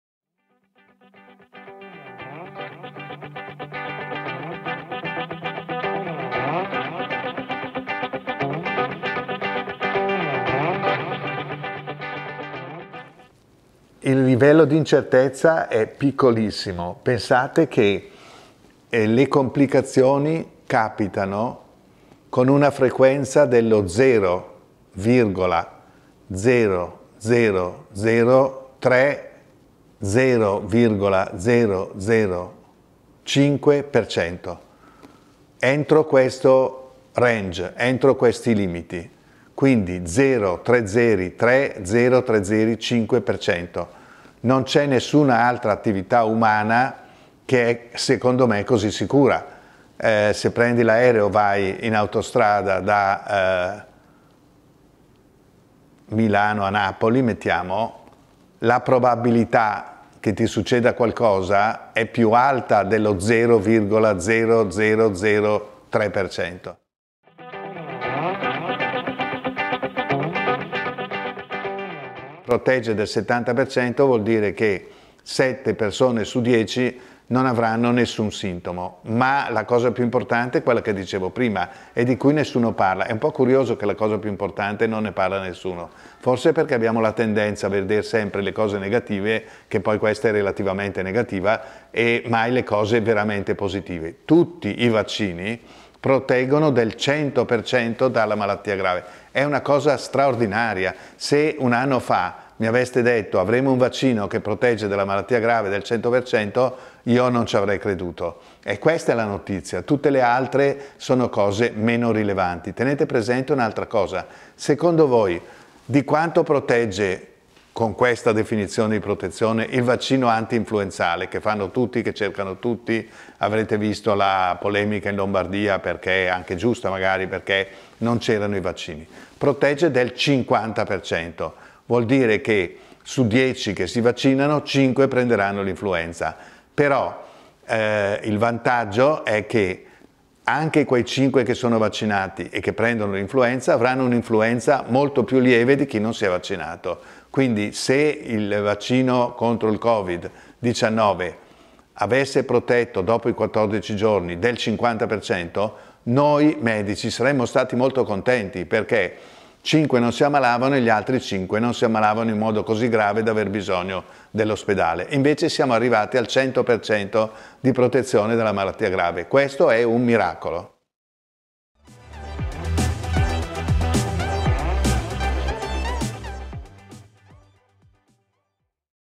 Pillola n°3 - IL VACCINO E' SICURO E PROTEGGE
Intervista al professor Giuseppe Remuzzi, direttore dell’istituto “Mario Negri”. Il prof. Remuzzi ci spiega in due minuti la sicurezza e la protezione dei vaccini - Pillola n°3